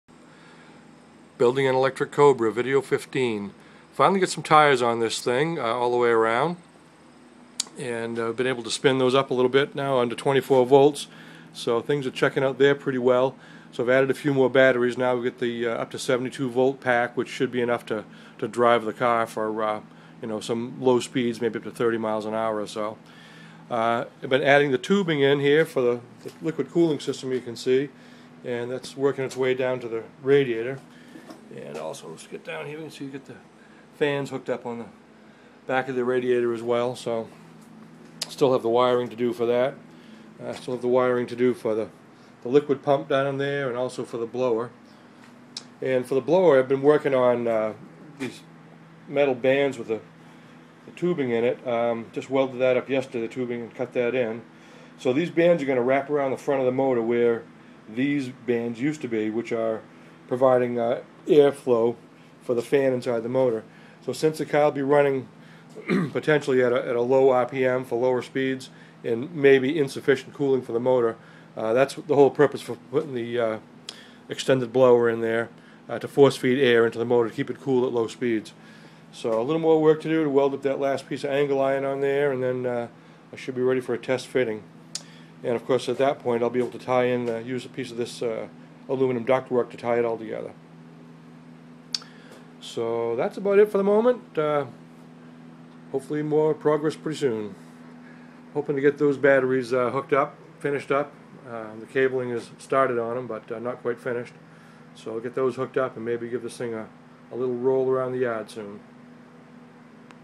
Factory Five Mk4 - Electric Car - Video 15
Building an electric Cobra, added wheels and tires, plumbing to the radiator, motor blower attachment rigging